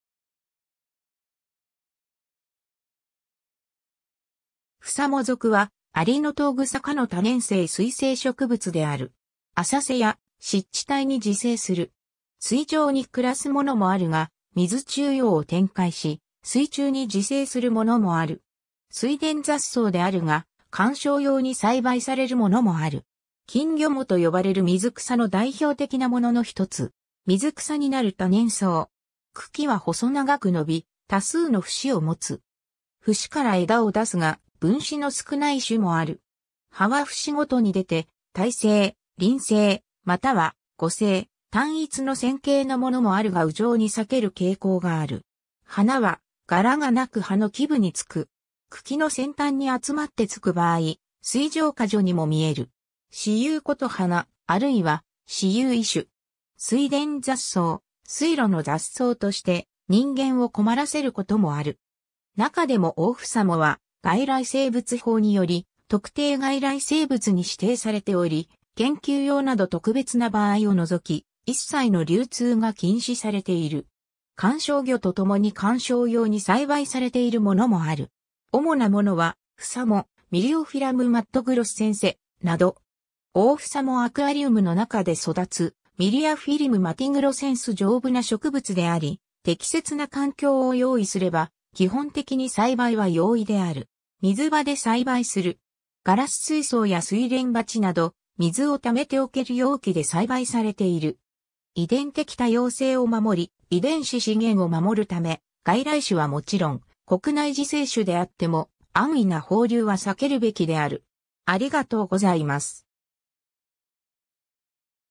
フサモ属
フサモ属（Myriophillum）は、アリノトウグサ科の多年生水生植物である。
浅瀬や湿地帯に自生する。
水上に暮らすものもあるが、水中葉を展開し、水中に自生するものもある。
水田雑草であるが、観賞用に栽培されるものもある。
金魚藻と呼ばれる水草の代表的なものの一つ。
 水草になる多年草。
茎は細長く伸び、多数の節を持つ。
節から枝を出すが、分枝の少ない種もある。
葉は節ごとに出て対生、輪生または互生、単一の線形のものもあるが羽状に裂ける傾向がある。
花は柄が無く葉の基部につく。
茎の先端に集まって着く場合、穂状花序にも見える。
雌雄異花、あるいは雌雄異株。
 水田雑草、水路の雑草として人間を困らせることもある。
中でもオオフサモは外来生物法により特定外来生物に指定されており、研究用など特別な場合を除き、一切の流通が禁止されている。
 観賞魚とともに観賞用に栽培されているものもある。
主なものは、フサモ、ミリオフィラム・マットグロッセンセ、など。
 オオフサモ（水上葉） アクアリウムの中で育つ Myriophyllum matogrossense （水中葉） 丈夫な植物であり、適切な環境を用意すれば、基本的に栽培は容易である。
 水場で栽培する。
ガラス水槽や睡蓮鉢など、水を貯めておける容器で栽培されている。
 遺伝的多様性を守り、遺伝子資源を守るため、外来種はもちろん、国内自生種であっても、安易な放流は避けるべきである。